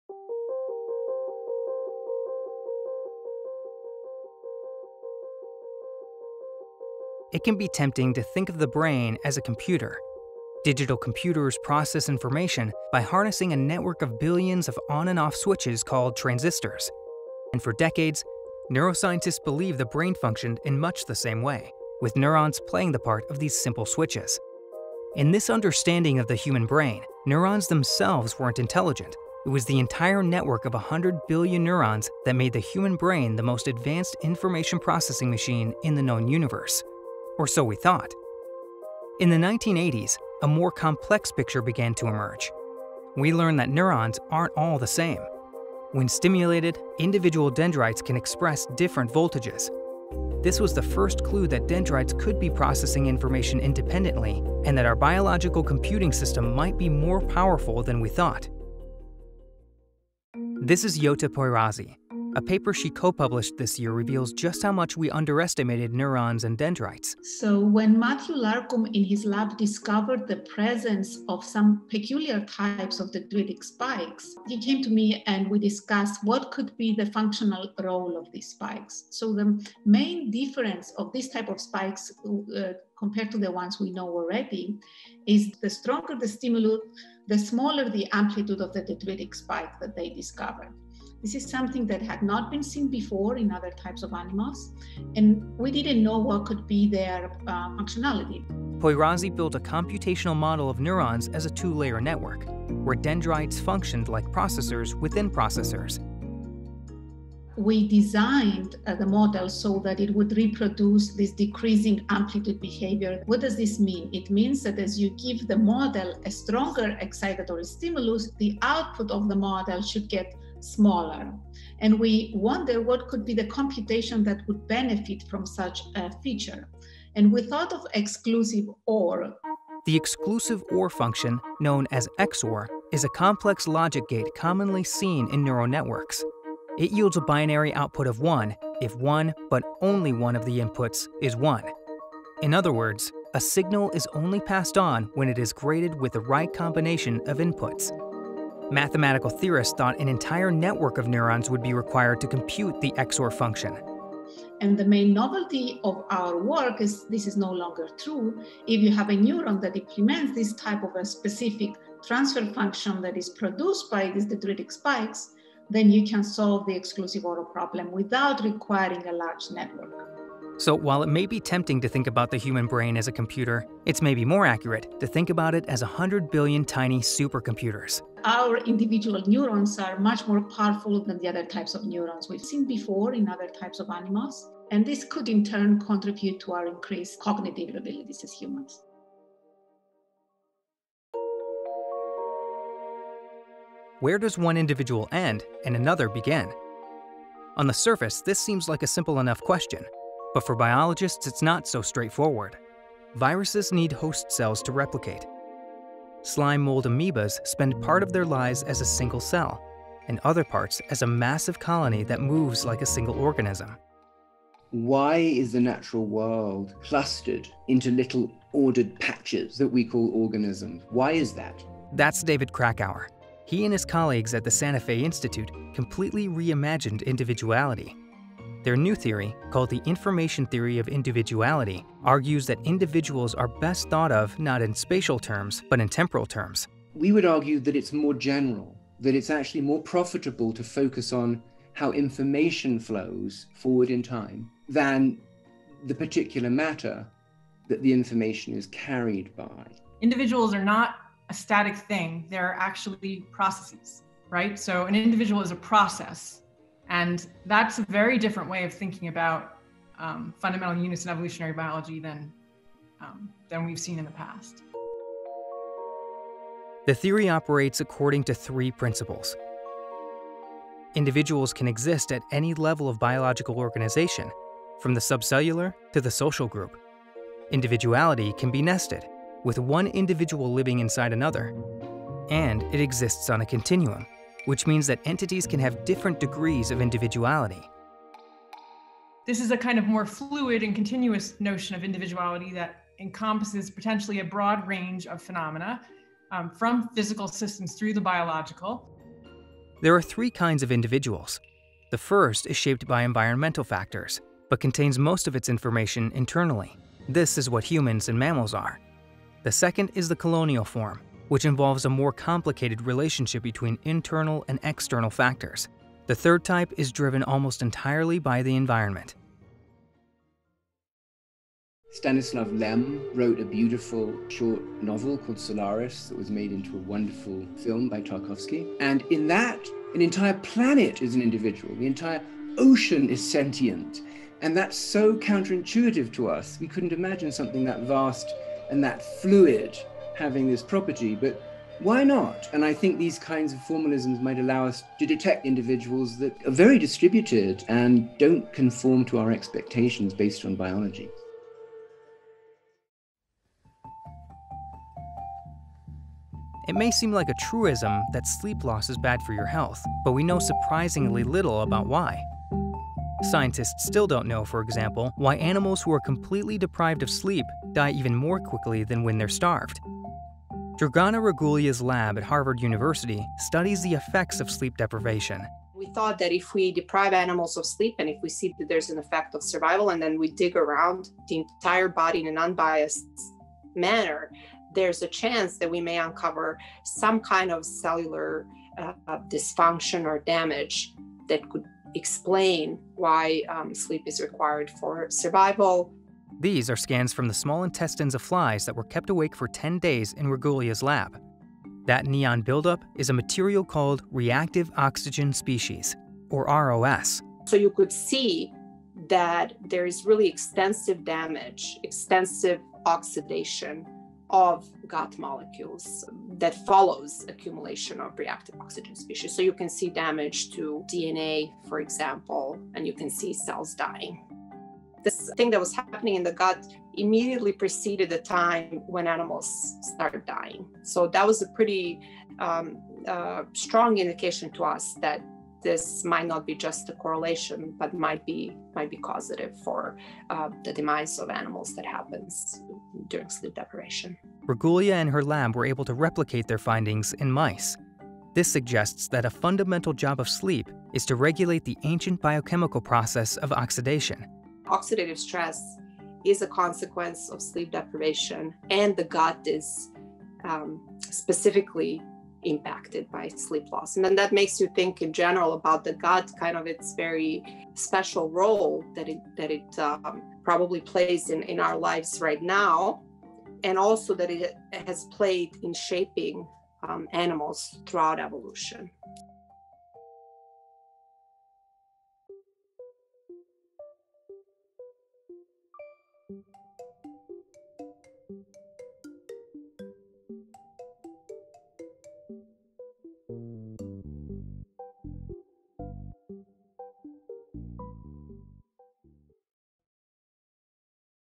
2020's Biggest Breakthroughs in Biology
In 2020, the study of the SARS-CoV-2 virus was undoubtedly the most urgent priority. But there were also some major breakthroughs in other areas. We'd like to take a moment to recognize them.

1. This year, we learned that we had severely underestimated the human brain's computing power. Researchers are coming to understand that even the dendritic arms of neurons seem capable of processing information, which means that every neuron might be more like a small computer by itself.

2. The new Information Theory of Individuality completely reimagines the way biologists have traditionally thought about individuality. Armed with information theory, the researchers found objective criteria for defining degrees of individuality in organisms.

3. Deprived of sleep, we and other animals die within weeks. More than a century of scrutiny failed to explain why lack of sleep is so deadly. This year, an answer was finally found — not inside the brain, as expected, but inside the gut.